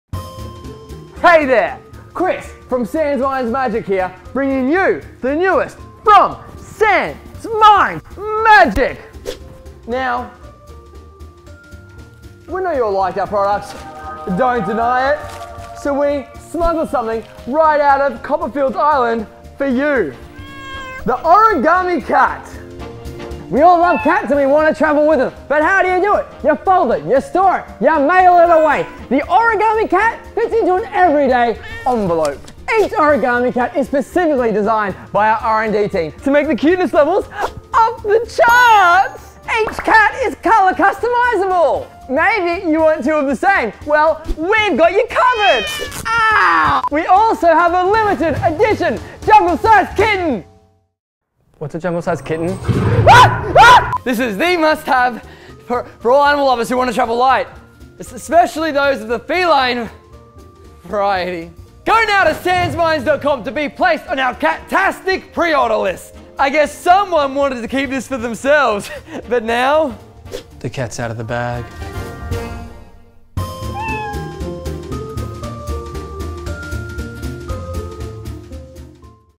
Cute Kitten That Folds
Note: No animals were harm during any part of the production of this video. (except Chris...but that is okay..)

A month ago we introduced you to 10 Magicians Pet Peeves.

Today we give you Project [K]. We have kept this project very secret for a long time now. Newest from SansMinds Magic!